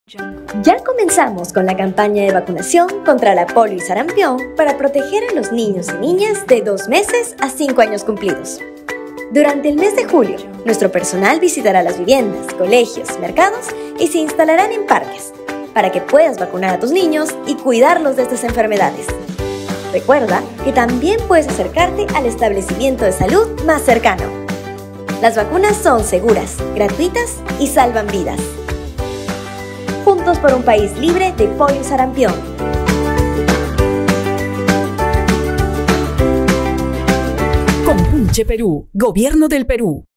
Diris Lima Sur inicia campaña de vacunación contra la polio y el sarampión
📣 ¡Atención, ya iniciamos la campaña de vacunación 💉 contra la polio y el sarampión! Durante todo julio nuestras brigadas 👨🏻‍⚕️👩🏻‍⚕️ se desplegarán en distintos puntos de LimaSur para vacunar a los pequeños de entre 2 meses a 5 años.
☝🏻 ¡Protégelos con amor, vacúnalos ya! 🥰